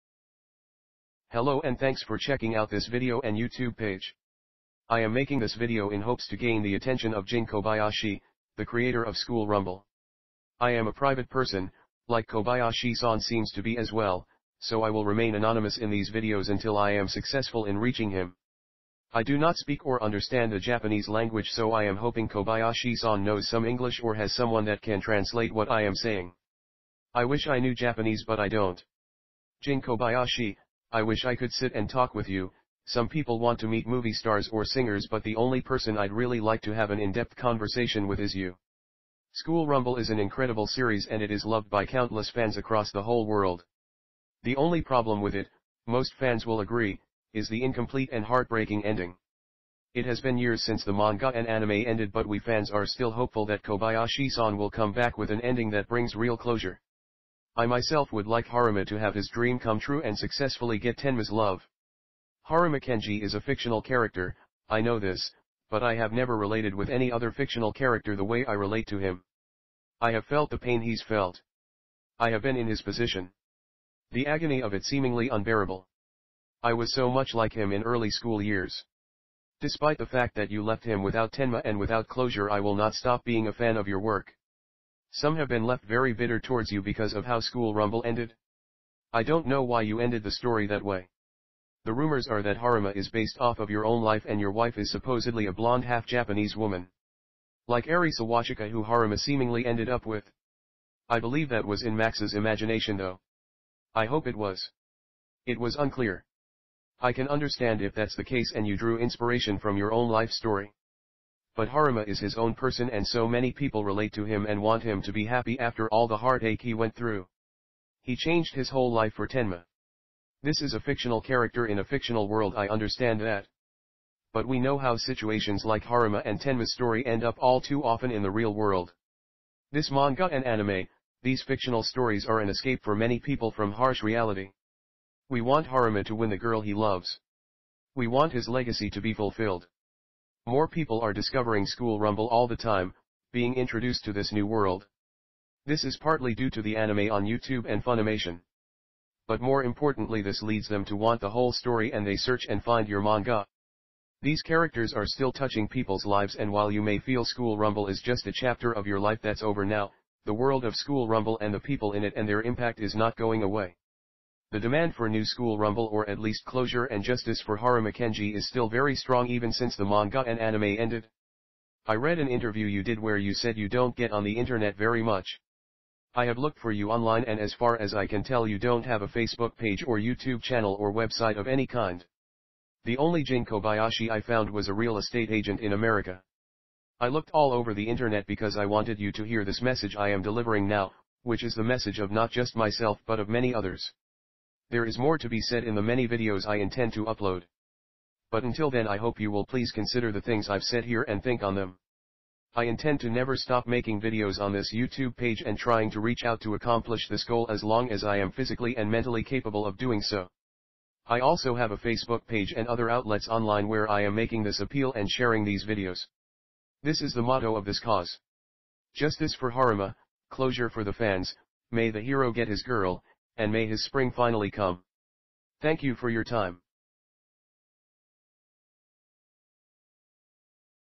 (UPDATED) Jin Kobayashi 小林 尽 mangaka please hear me 私の言う事を聞いて下さい。 UPDATED
UPDATED 12-27-14

私の言う事を聞いて下さい。

Hello and thanks for checking out this video and YouTube page. I am making this video in hopes to gain the attention of Jin Kobayashi, the creator of School Rumble. I am a private person, like Kobayashi-san seems to be as well, so I will remain anonymous in these videos until I am successful in reaching him.  I do not speak or understand the Japanese language so I am hoping Kobayashi-san knows some English or has someone that can translate what I am saying. I wish I knew Japanese but I don't.
 Jin Kobayashi, I wish i could sit and talk with you,Some people want to meet movie stars or singers but the only person I'd really like to have an in depth conversation with is you. School Rumble is an incredible series and it is loved by countless fans across the whole world. The only problem with it, most fans will agree, is the incomplete and heartbreaking ending. It has been years since the manga and anime' ended but we fans are still hopeful that Kobayashi-san will come back with an ending that brings real closure. I myself would like Harima to have his dream come true and successfully get Tenma's love. Harima Kenji  is a fictional character, I know this, but i have never related with any other fictional character the way i relate to him. I have felt the pain he's felt. I have been in his position. The agony of it seemingly unbearable. I was so much like him in early school years. Despite the fact that you left him without Tenma and without closure I will not stop being a fan of your work.
 Some have been left very bitter towards  you because of how School rumble ended. I don't know why you ended   the story  that way. The rumors are that Harima is based off of your own life and your wife is supposedly a blonde half Japanese woman,
 like Eri Sawachika who Harima seemingly ended up with. I believe that was in Max's imagination though.  I hope it was. It was unclear. I can understand if that's the case and you drew inspiration from your own life story. But Harima is his own person and so many people relate to him and want him to be happy after all the heart ache he went through. He changed his whole life for Tenma. This is a fictional character in a fictional world I understand that. But we know how situations like Harima and Tenma's story end up all too often in the real world. This manga and anime', these fictional stories are an escape for many people from harsh reality. We want Harima to win the girl he loves. We want his legacy to be fulfilled. More people are discovering School Rumble all the time, being introduced to this new world. This is partly due to the anime' on YouTube and Funimation. But more importantly this leads them to want the whole story and they search and find your manga.
These characters are still touching people's lives and while you may feel School Rumble is just a chapter of your life that's over now, the world of School Rumble and the people in it and their impact is not going away. The demand for new School Rumble or at least closure and justice for Harima Kenji is still very strong even since the manga and anime' ended. I red an interview you did where you said you don't get on the internet very much. I have looked for you online and as far as I can tell you don't have a Facebook page or YouTube channel or website of any kind. The only Jin Kobayashi I found was a real estate agent in America. I looked all over the internet because I wanted you to hear this message I am delivering now, which is the message of not just myself but of many others. There is more to be said in the many videos I intend to upload. But until then I hope you will please consider the things I've said here and think on them.
 I intend to never stop making videos on this YouTube page and trying to reach out to accomplish this goal as long as I am physically and mentally capable of doing so. I also have a Facebook page and other outlets online where i am making this appeal and sharing these videos.
This is the motto of this cause,..  justice for Harima, closure for the fans, may the hero get his girl, and may his spring finally come.